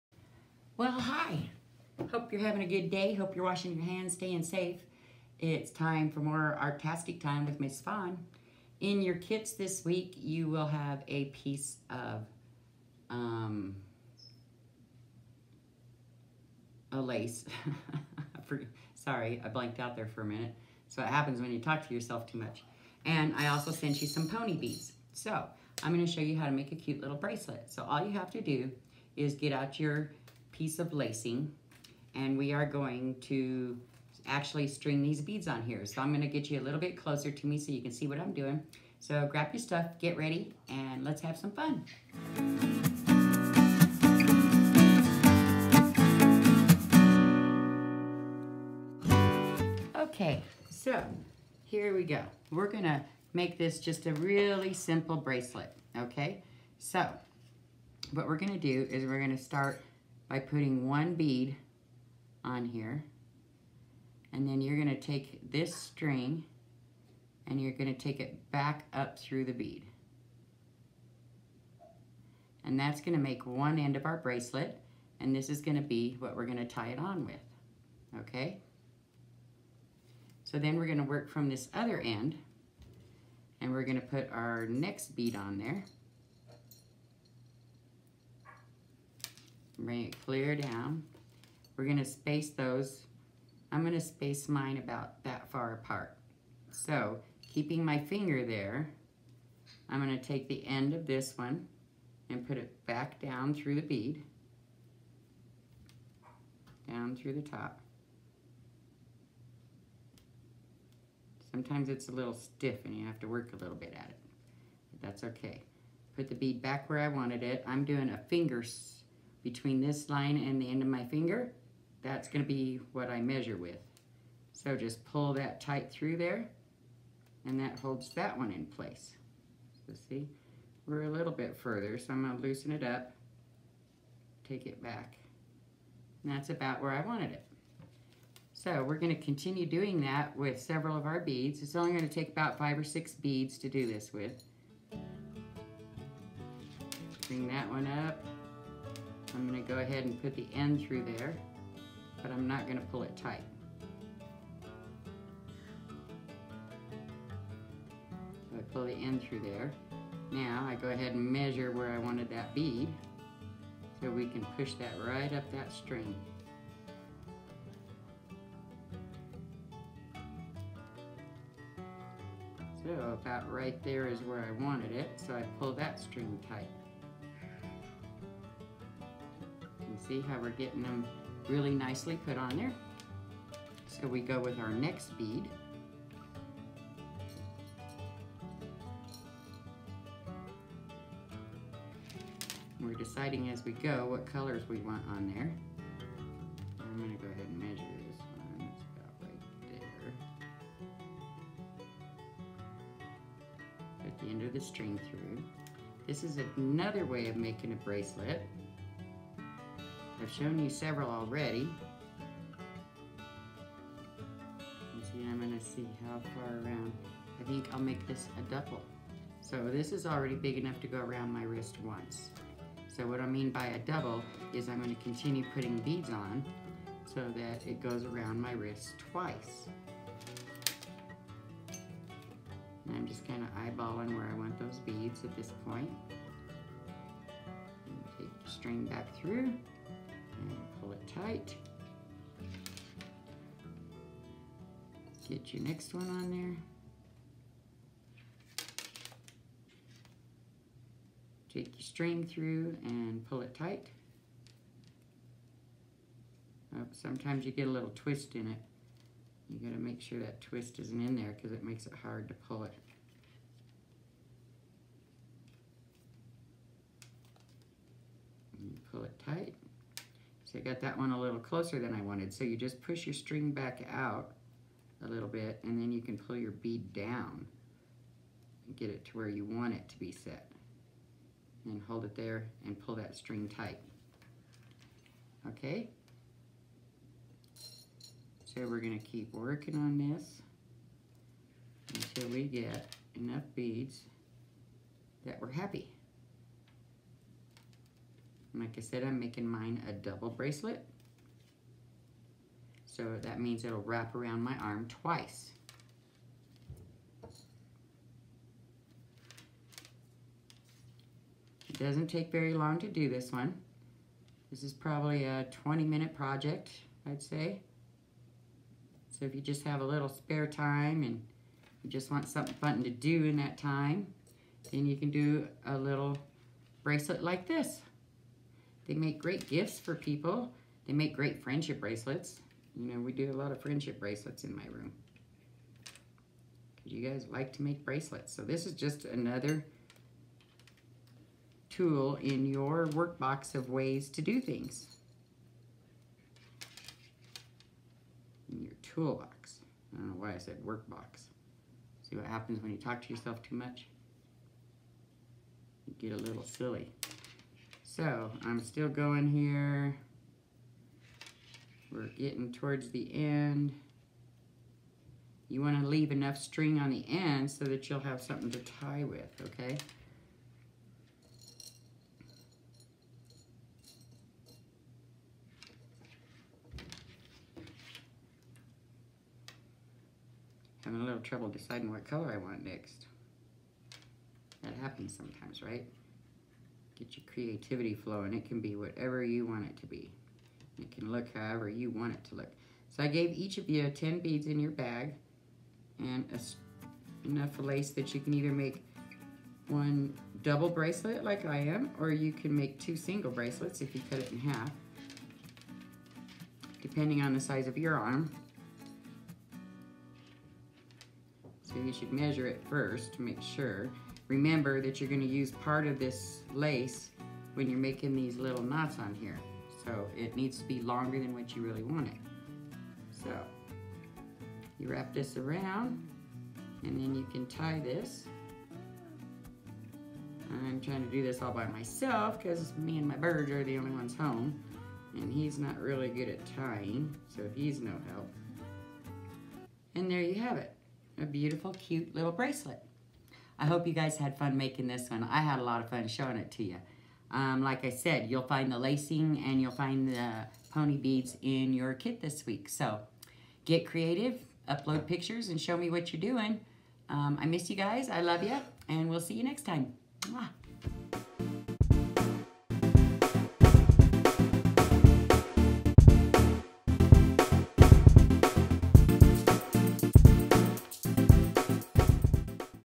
Art-Tastic w/Ms.Fawn:Plastic Lace Bracelet
Happy Friday! Here’s an easy bracelet project that doesn’t take long. Grab the piece of plastic lace and the pony beads from your kits and let’s make this fun, colorful bracelet!